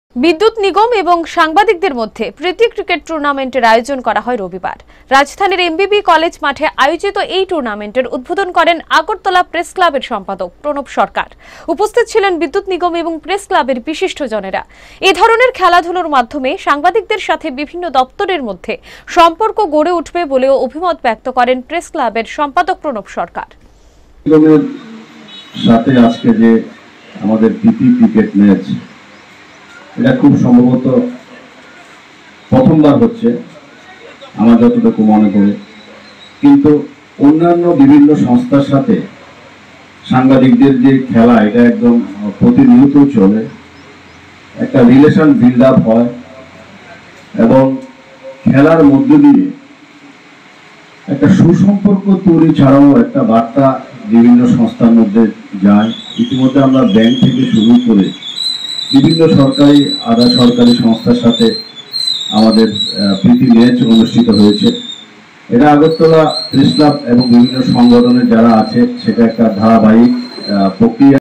বিদ্যুৎ নিগম এবং সাংবাদিকদের মধ্যে প্রীতি ক্রিকেট টুর্নামেন্টের আয়োজন করা হয় রবিবার
বিদ্যুৎ নিগম এবং সাংবাদিকদের মধ্যে প্রীতি ক্রিকেট টুর্নামেন্টের আয়োজন করা হয় রবিবার। রাজধানীর এমবিবি কলেজ মাঠে আয়োজিত এই টুর্নামেন্টের উদ্বোধন করেন আগরতলা প্রেসক্লাবের সম্পাদক প্রণব সরকার। উপস্থিত ছিলেন বিদ্যুৎ নিগম এবং প্রেসক্লাবের বিশিষ্ট জনেরা। এধরনের খেলাধুলার মাধ্যমে সাংবাদিকদের সাথে বিভিন্ন দপ্তরের মধ্যে সুসম্পর্ক গড়ে উঠবে বলেও অভিমত ব্যক্ত করেন প্রেসক্লাবের সম্পাদক প্রণব সরকার।